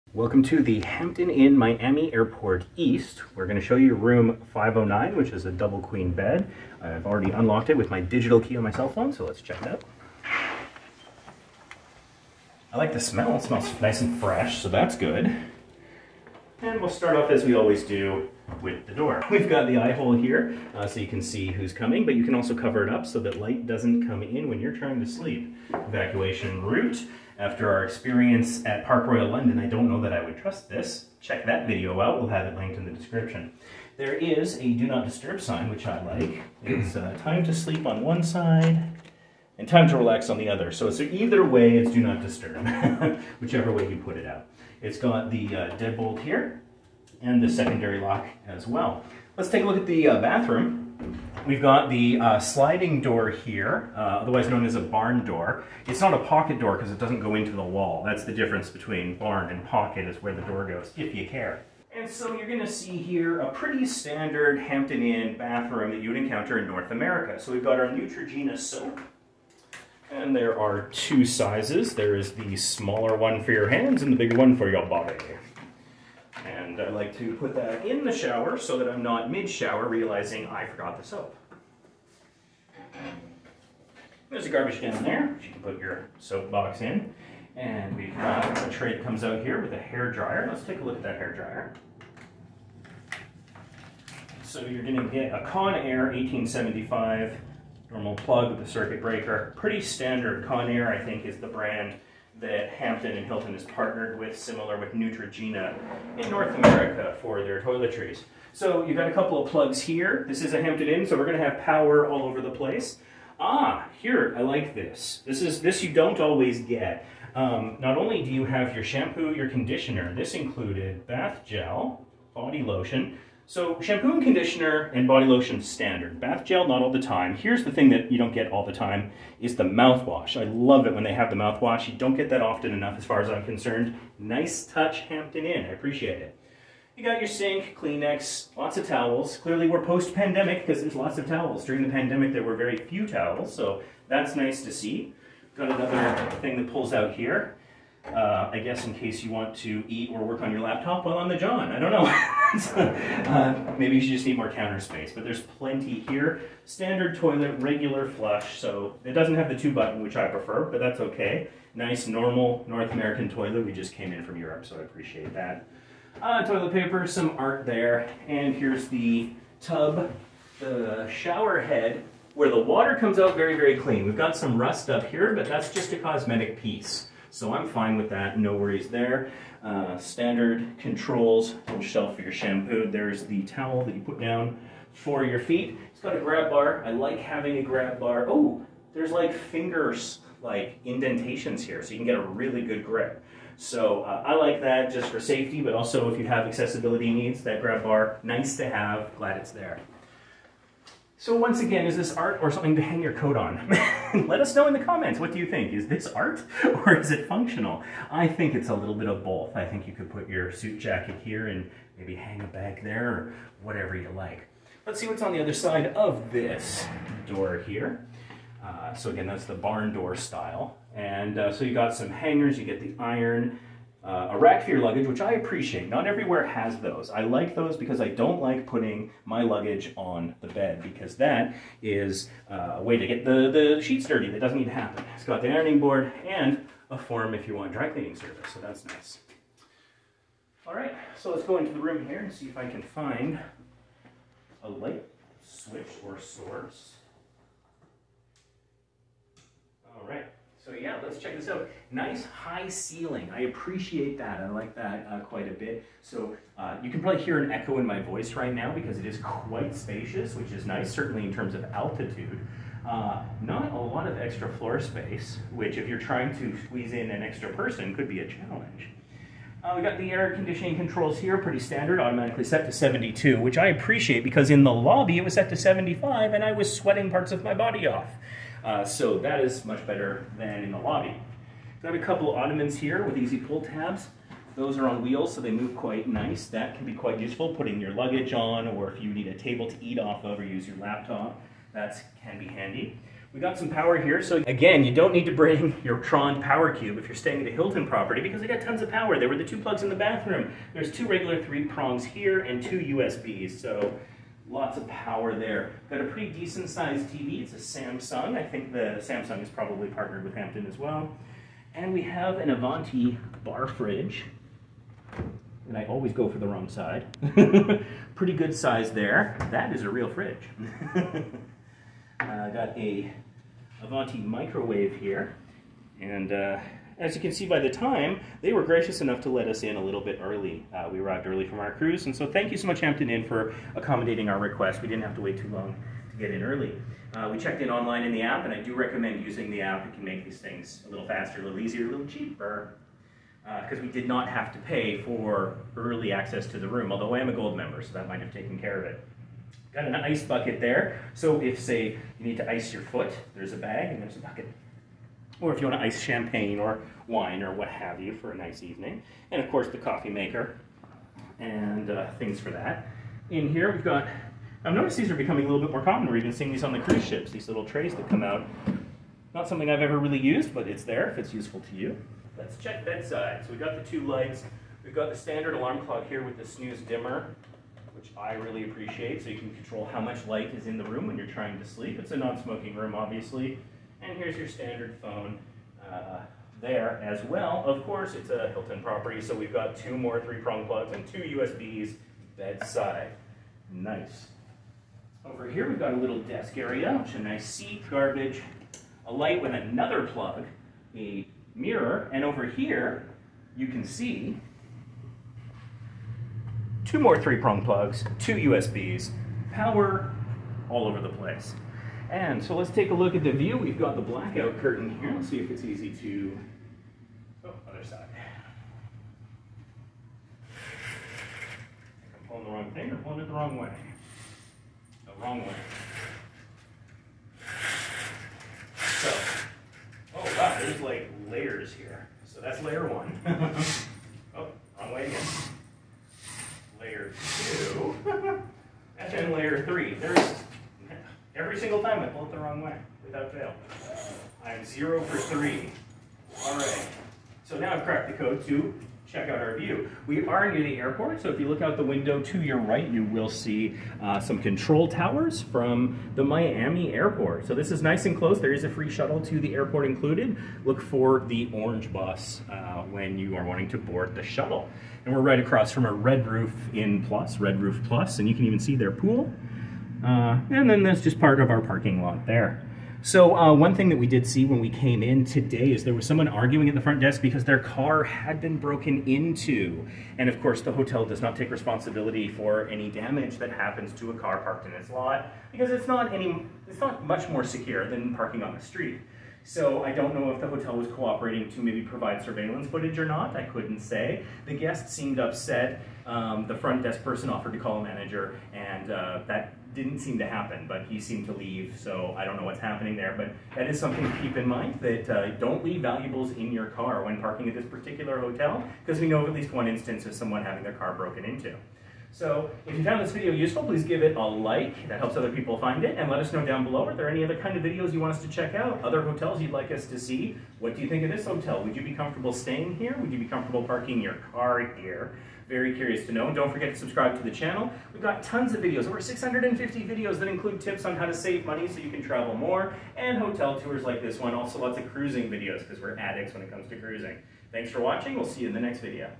Hampton Inn Miami FL Airport East
Hampton Inn Miami FL Airport East. If you're wondering where to stay before your cruise out of Miami this hotel is a good option.

Hampton Inn Miami Airport East Location: 3449 NW 42nd Ave. Miami FL 33142 USFeaturing a year-round outdoor pool and fitness center, which unfortunately we didn't have time to try, and the rainy weather didn't really make us miss the pool. Hampton Inn Miami Airport East is located in Miami, the cruise capital of the world making this a potentially ideal pre-cruise hotel.Guestrooms include a flat-screen TV, coffee-making facilities, and a private bathroom. We're told select rooms offer a microwave and small refrigerator - ours did not.Complimentary breakfast is provided for guests of Hampton Inn Miami Airport East. Guests can use the launderette, luggage storage, and business center during their stay.Miami's city center is 7.5 mi from the hotel while Miami International Airport is 2.5 mi away. The hotel offers a free shuttle to and from the airport.

This hotel has TONS of power outlets so if you decide to stay in a non-Hilton hotel property instead we strongly suggest bringing one of these:
Trond Prime Nano Travel Power Strip w/USB

VIDEO TIMESTAMPS - Hampton Inn Miami FL Airport East
00:00 Entrance
01:01 Bathroom
03:20 Room
06:53 View